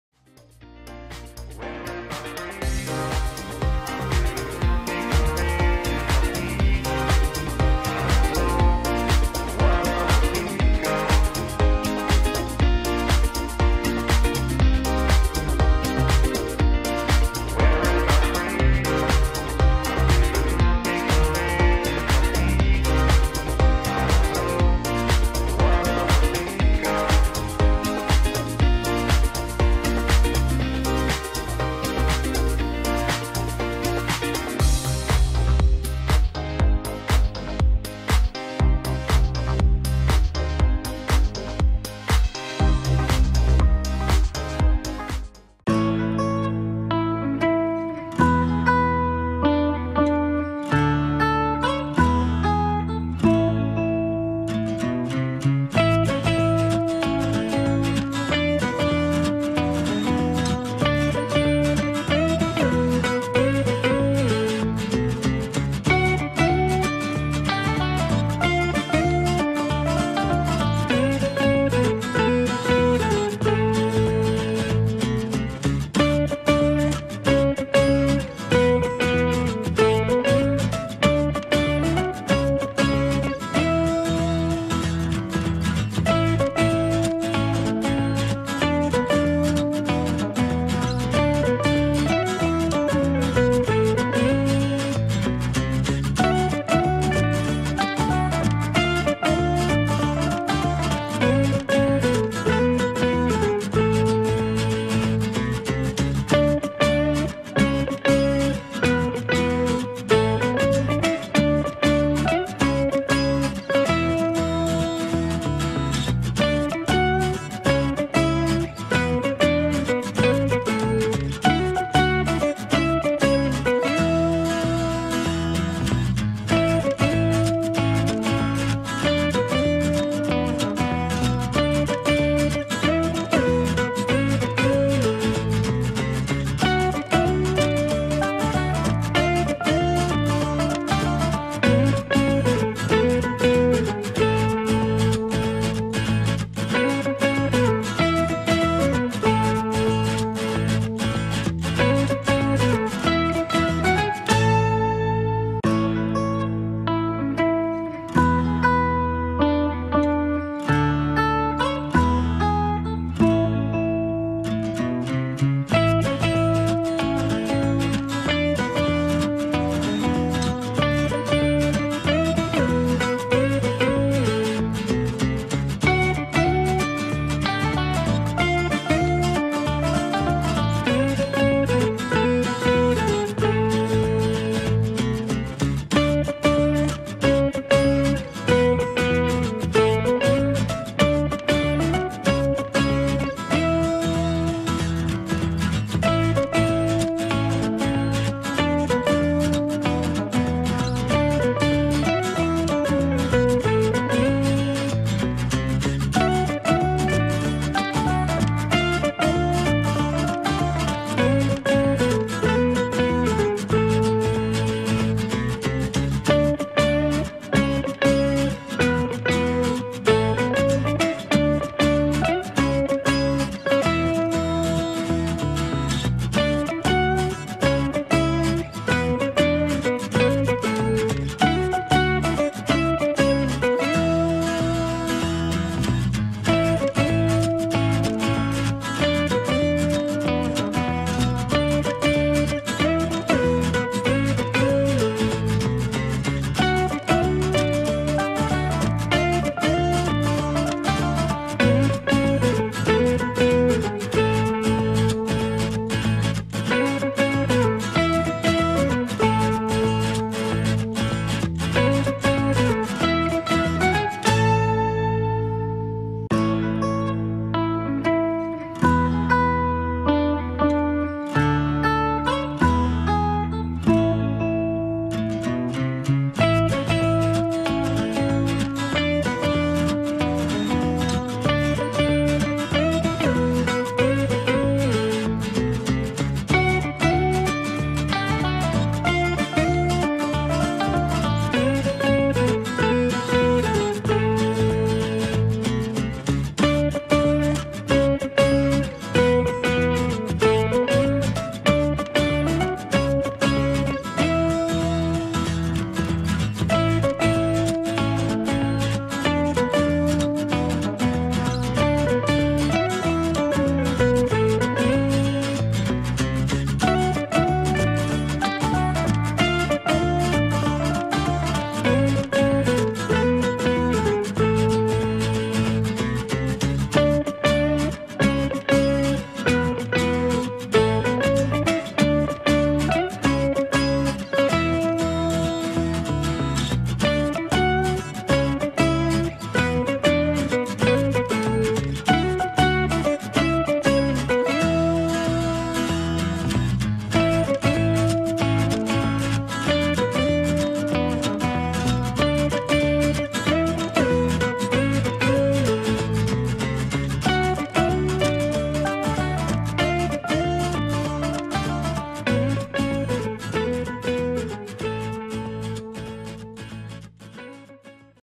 Tube well in Pakistan | Swimming and Jumping in Tube well Water | Solar tube well | chamkeela
Always love to jump in the water Tube well in Pakistan . Best Swimming and Jumping in Tube well Water, Are you know about Solar tube well.